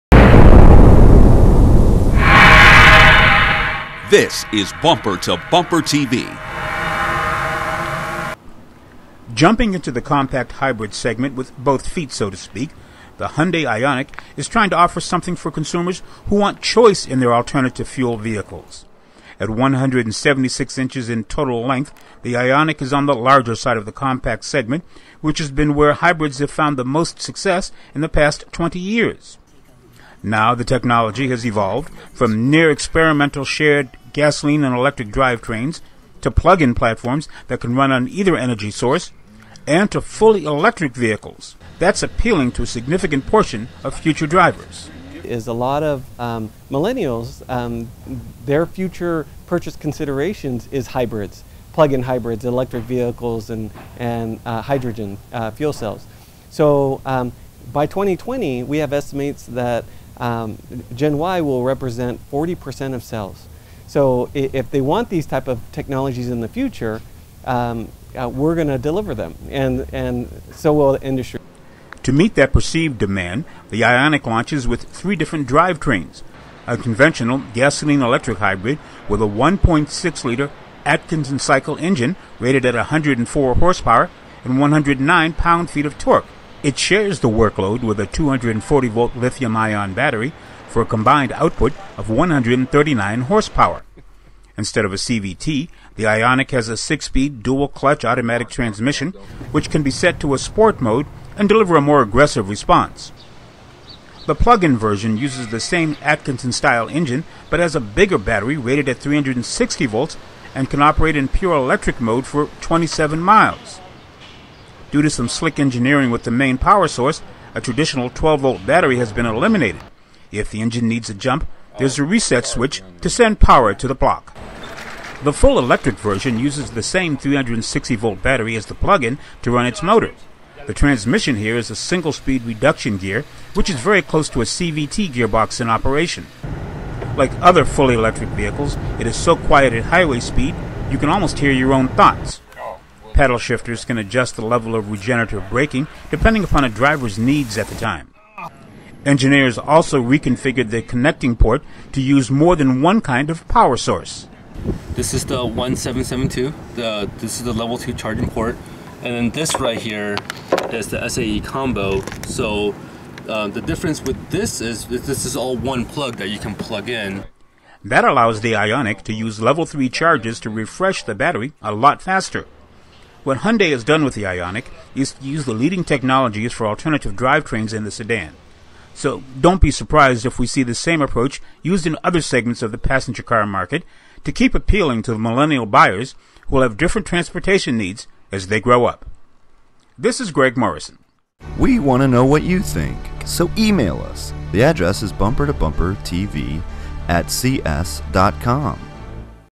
HYUNDAI IONIQ, A FIRST LOOK!!!
Hyundai has developed a different platform to attract drivers who like hybrid and alternative fuel vehicles. Bumper2Bumpertv has a first look at the Ioniq, which offers 3 drive trains for hybrid drivers.